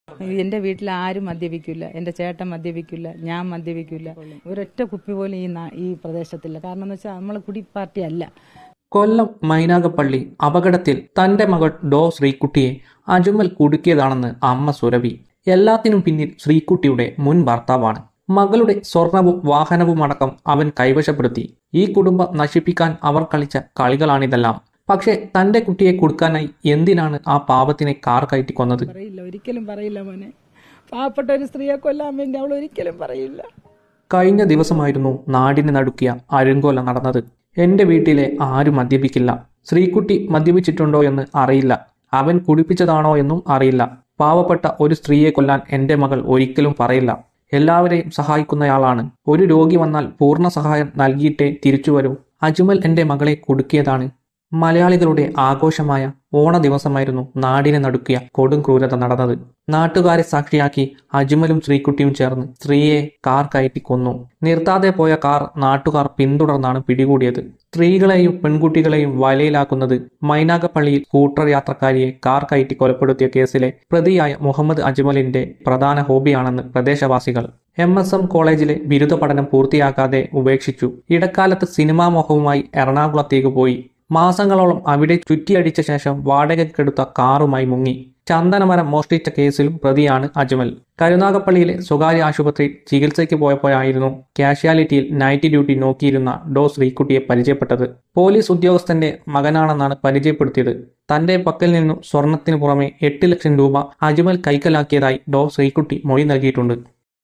നാട്ടുകാർ വരെ മദ്യം കുടിക്കാറില്ല 🙏 dr sreekutty amma about | ajmal
കൊല്ലം മൈനാഗപ്പള്ളി അപകടത്തിൽ തന്റെ മകൾ ഡോ.ശ്രീക്കുട്ടിയെ അജ്മൽ കുടുക്കിയതാണെന്ന് അമ്മ സുരഭി. എല്ലാത്തിനും പിന്നിൽ ശ്രീക്കുട്ടിയുടെ മുൻ ഭർത്താവാണ്. മകളുടെ സ്വർണവും വാഹനവും അടക്കം അവൻ കൈവശപ്പെടുത്തി. ഈ കുടുംബം നശിപ്പിക്കാൻ അവർ കളിച്ച കളികൾ ആണിതെല്ലാം. പക്ഷേ തന്റെ കുട്ടിയെ കുടുക്കാനായി എന്തിനാണ് ആ പാവത്തിനെ കാർ കയറ്റി കൊന്നത്? കഴിഞ്ഞ ദിവസമായിരുന്നു നാടിന് നടുക്കിയ ആരും കൊല നടന്നത്  എന്റെ വീട്ടിലെ ആരും മദ്യപിക്കില്ല. ശ്രീക്കുട്ടി മദ്യപിച്ചിട്ടുണ്ടോയെന്ന് അറിയില്ല. അവന്‍ കുടിപ്പിച്ചതാണോയെന്നും അറിയില്ല. പാവപ്പെട്ട ഒരു സ്ത്രീയെ കൊല്ലാന്‍ എന്റെ മകള്‍ ഒരിക്കലും പറയില്ല